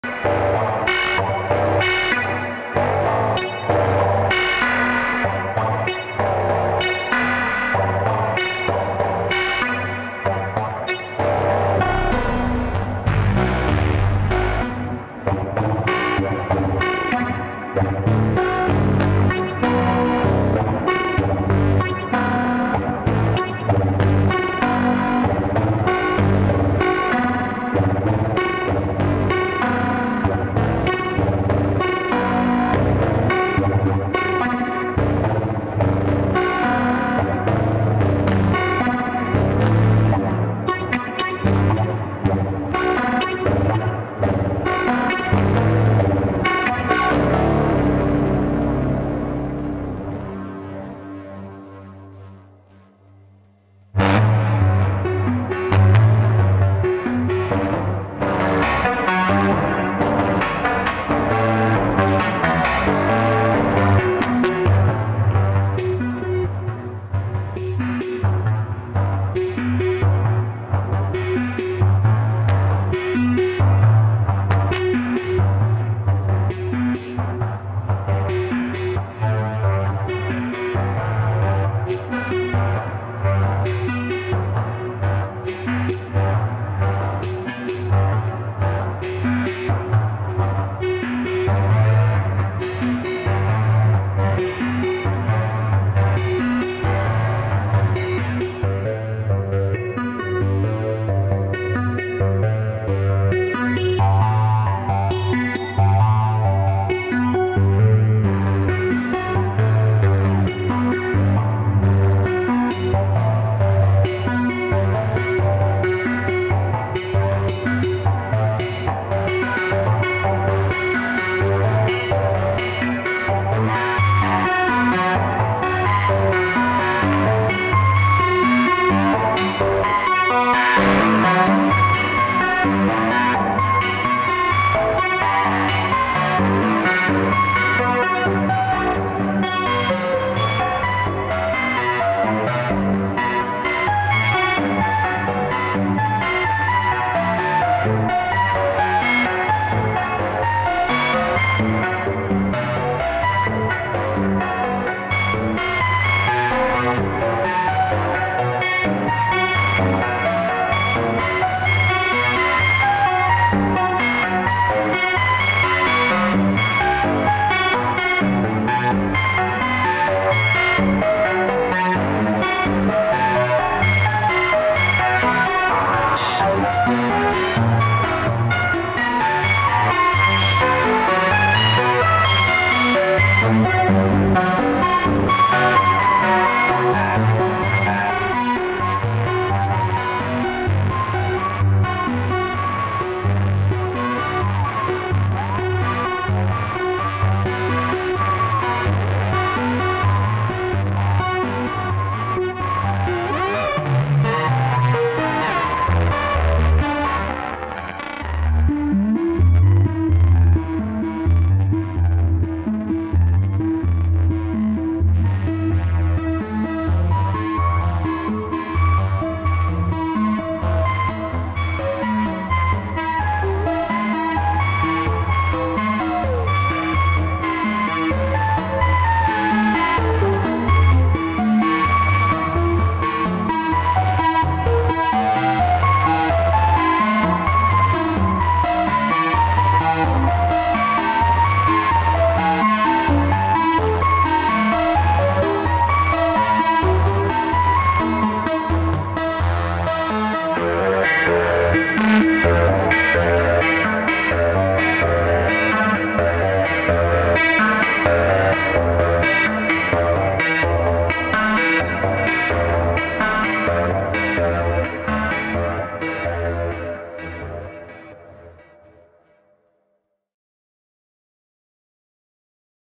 Feedback // Make Noise 0-Coast and Sleepy Circuits Hypno
Some more jamming with the 0-Coast and Sleepy Circuits Hypno video synthesizer.